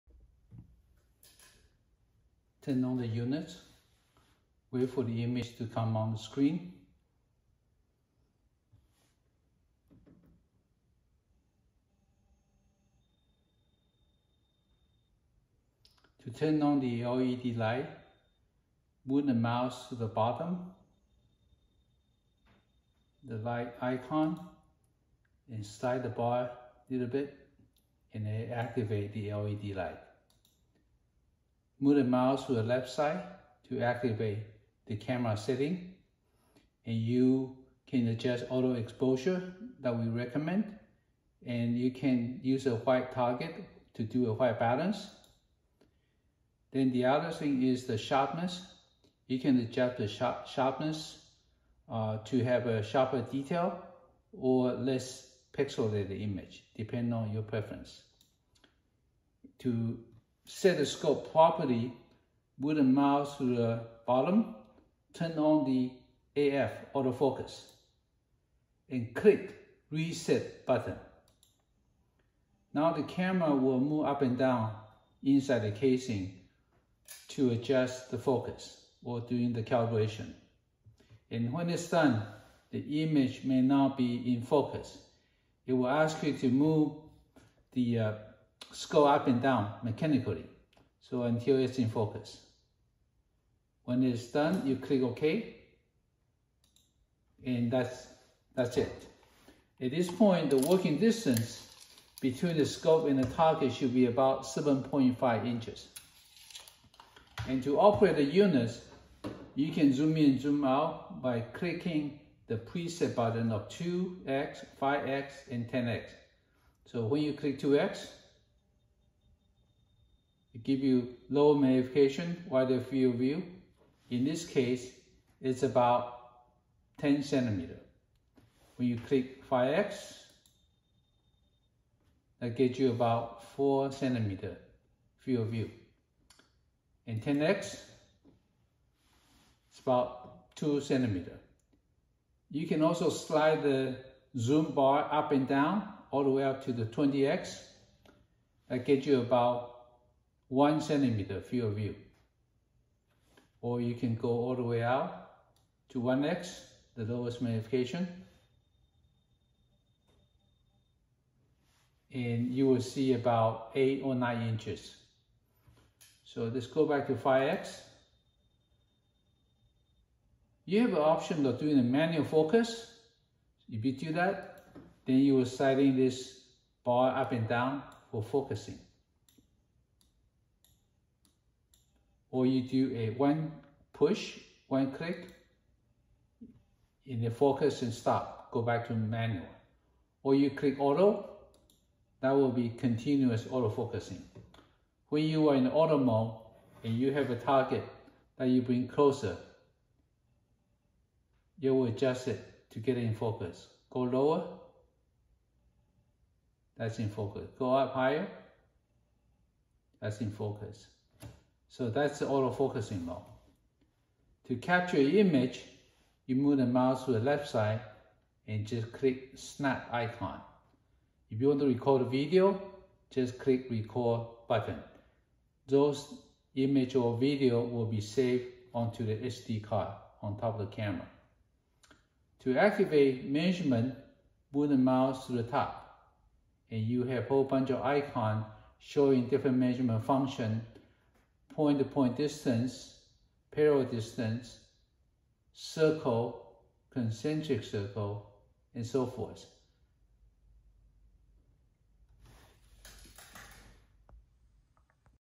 How to Operate Caltex AX-4K Auto-Focus HD Microscope Operation
Inspecting from 200 mm to 10 mm field of view with auto-focus HD camera and motorized zoom.

Turning on the Camera
Turning on LED Ring Light
Camera Menu Setting
Turning on Auto-Focus
Calibrate the Scope
Zooming in and out to different magnifications
Using Auto or Manual Focusing
Capture Photos
Activating Measurement Functions